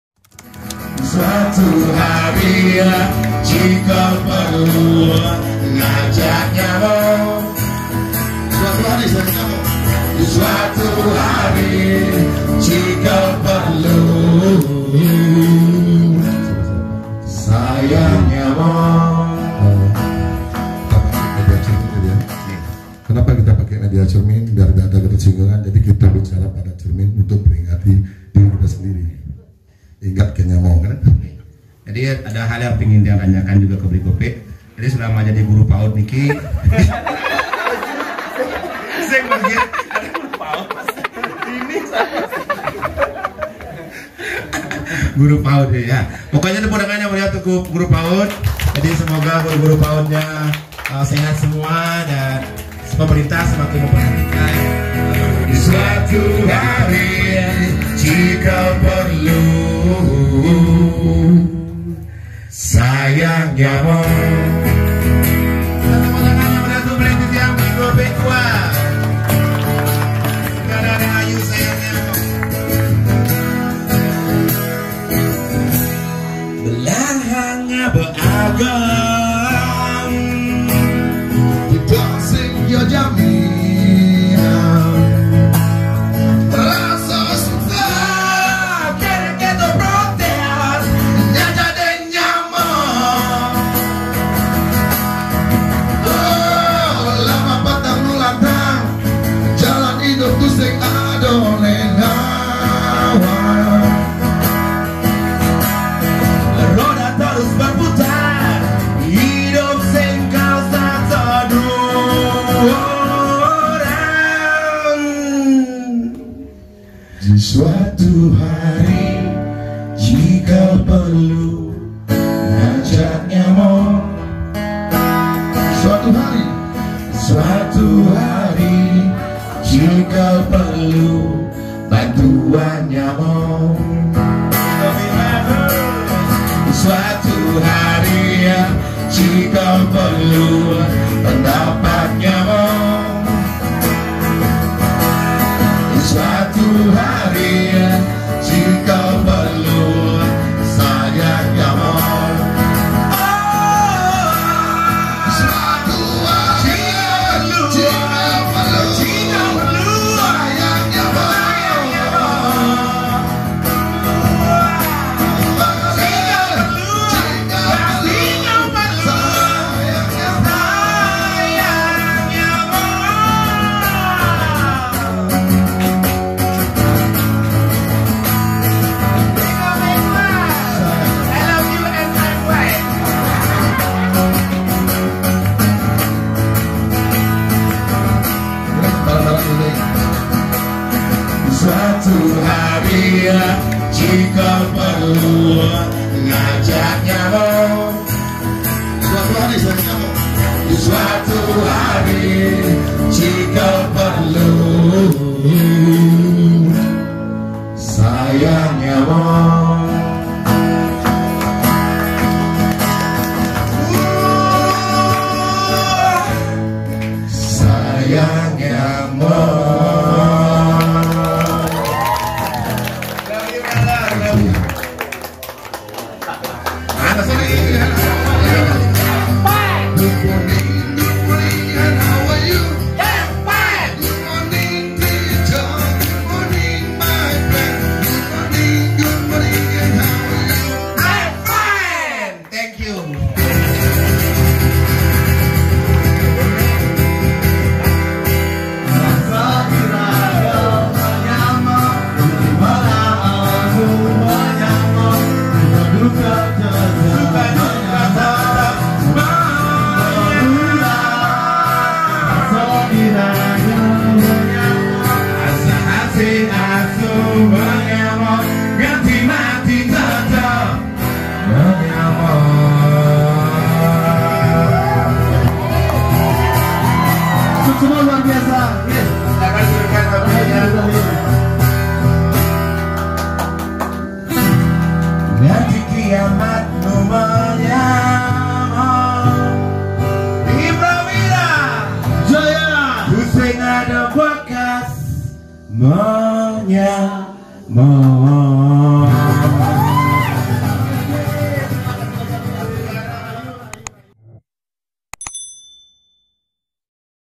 SAYANG NYAMA Bersama Guru-guru PAUD @ HIMPRAWIRA 2021
Senang hati "I'm Fine" menghibur guru-guru PAUD/TK 🤟😍
Dumogi rahayu sareng sami Keluarga besar HIMPRAWIRA (Himpunan Pendidik dan Tenaga Kependidikan Pratama Widya Pasraman) 🌱📖❤🙏🏼